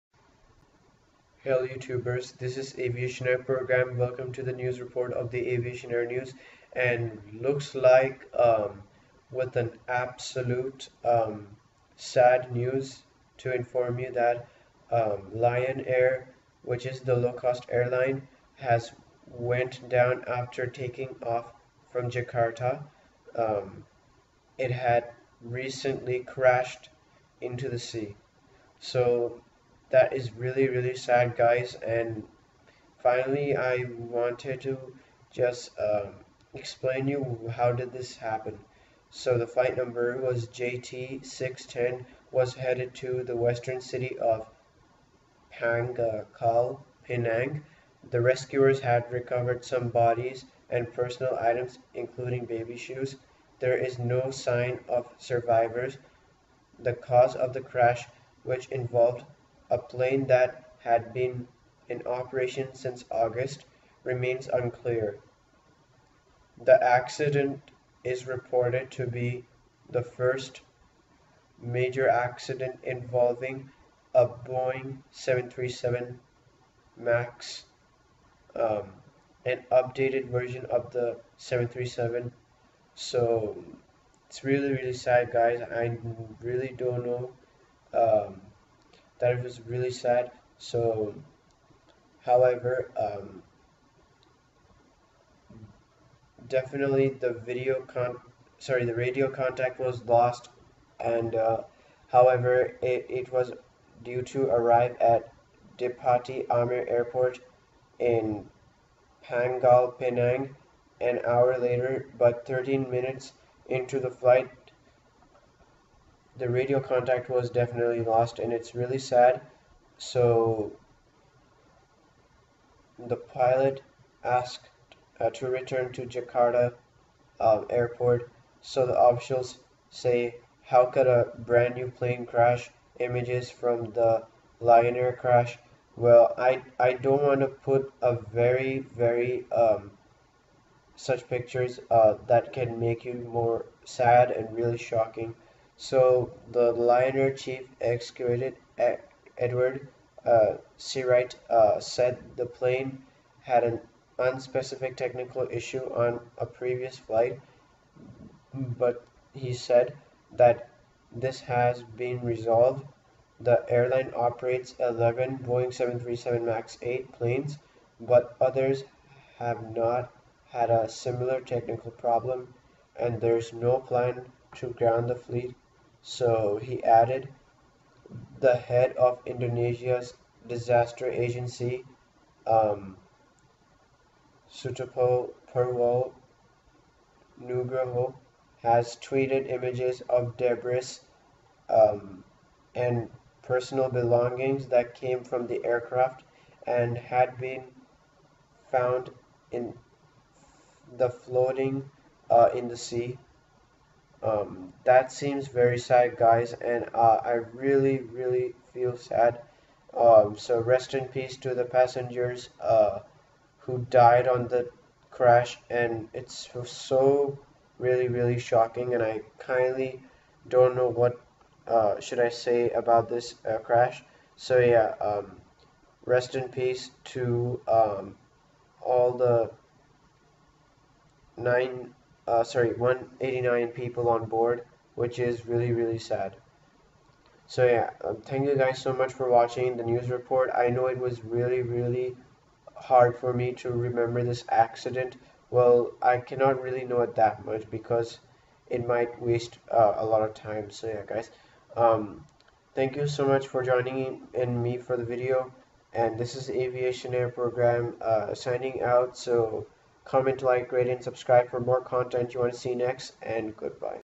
Lion Air Boeing 737 Crashed Into The Sea | NEWS REPORT
Hey guys Aviation Air Program here and this news report with an absolute devastating news information about Lion Air
(a low cost airline) had crashed into the sea after taking off from Jakarta the airline operated a Boeing 737 Max 8.